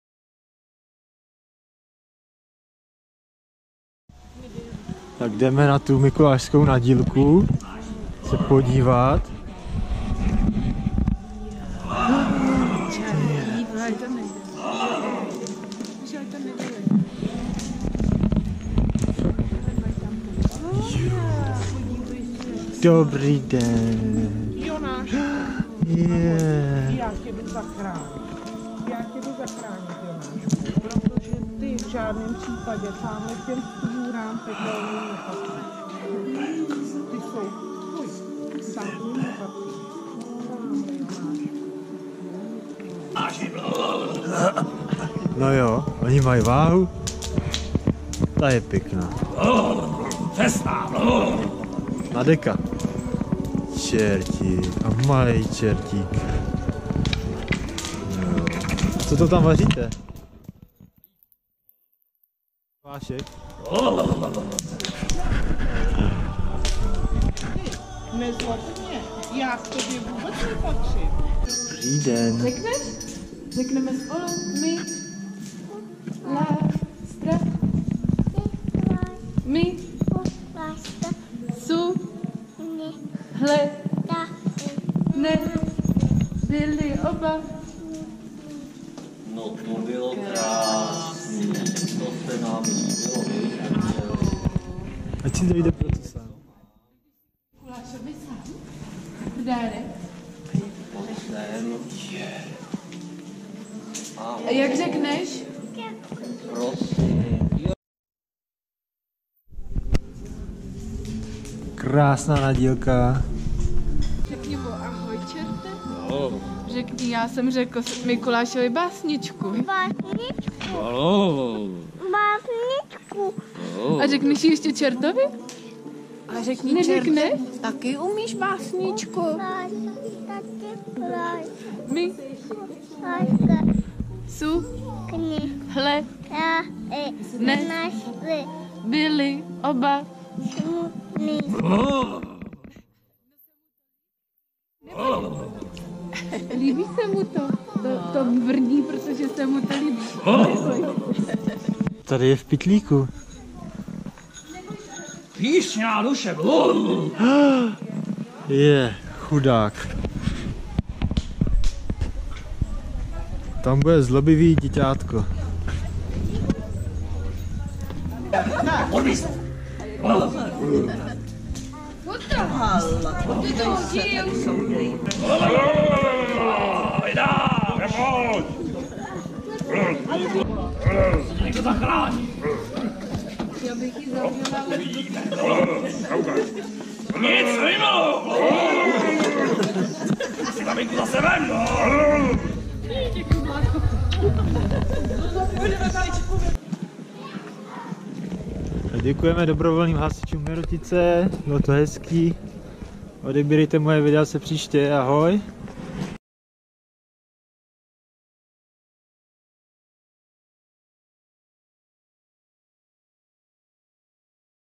Mikuláš Anděl a Čerti s SDH Mirotice: peklo v hasičárně 2019
Nebe a peklo v podání dobrovolných hasičů Mirotice
Dvouletý Jonášek poprvé říká básničku
Vánoce 2019